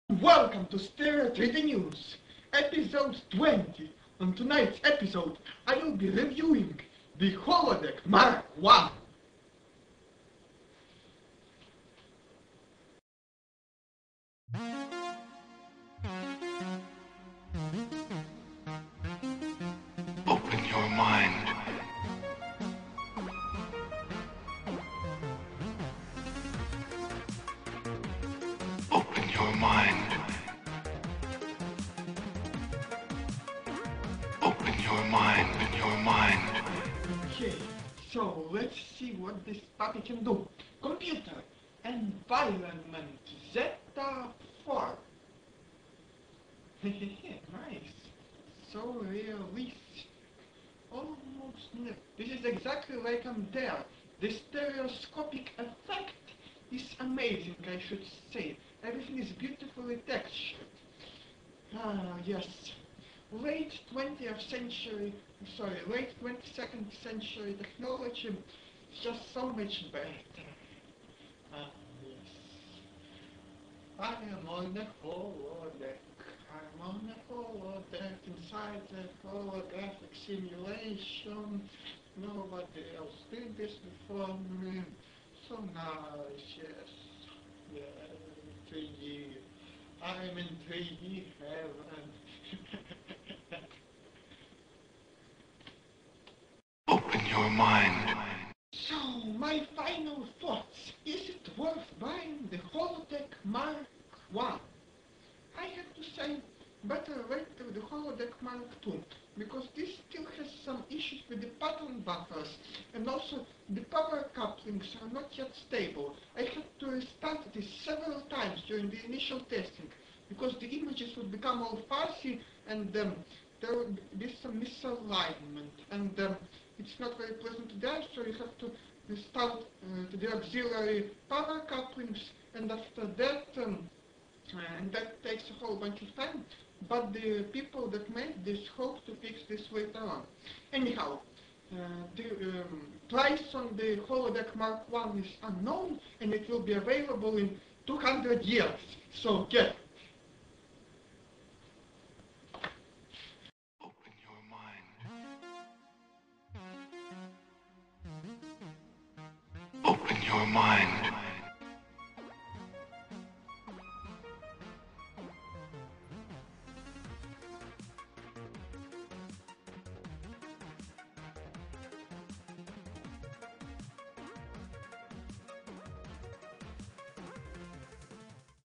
stereo3dnews: Holodeck Mark1 review (stereopair)
The holodeck mark one has many features, however the axillary pattern buffers remain an issue.